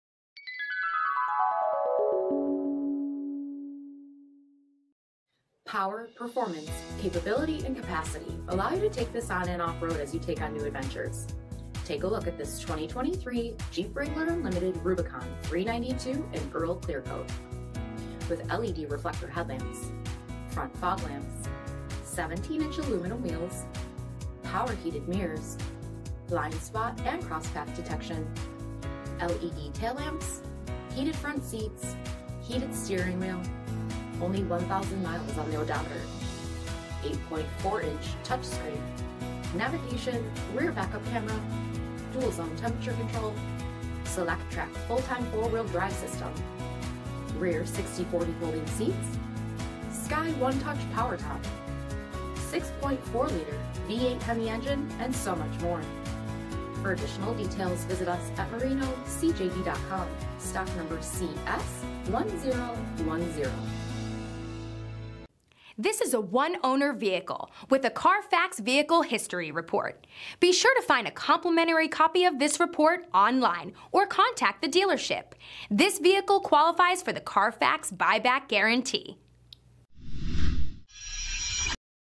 2023 Jeep Wrangler 90003A
2023 Jeep Wrangler Rubicon 392

5133 W Irving Park Rd

2023 JEEP WRANGLER RUBICON 392!!  6.4 LITER V8 HEMI ENGINE!!  STILL UNDER FACTORY BUMPER TO BUMPER AND POWERTRAIN WARRANTY!!  ONE OWNER!!  ACCIDENT FREE CARFAX REPORT!!  17 INCH ALUMINUM WHEELS!!  SKY ONE TOUCH POWER TOP!!  8.4 INCH TOUCH SCREEN DISPLAY!!  NAVIGATION!!  REAR BACKUP CAMERA!!  ALPINE PREMIUM AUDIO SYSTEM!!  STEEL BUMPER GROUP!!  COLD WEATHER GROUP!!  HEATED FRONT SEATS!!  HEATED STEERING WHEEL!!  REMOTE START SYSTEM!!  POWER HEATED MIRRORS!!  LED LIGHTING GROUP!!  STEERING WHEEL MOUNTED SHIFT CONTROL!!  APPLE CARPLAY!!  GOOGLE ANDROID AUTO!!  SIRIUSXM RADIO!!  ELECTRONIC STABILITY CONTROL!!  POWER LOCKS!!  POWER WINDOWS!!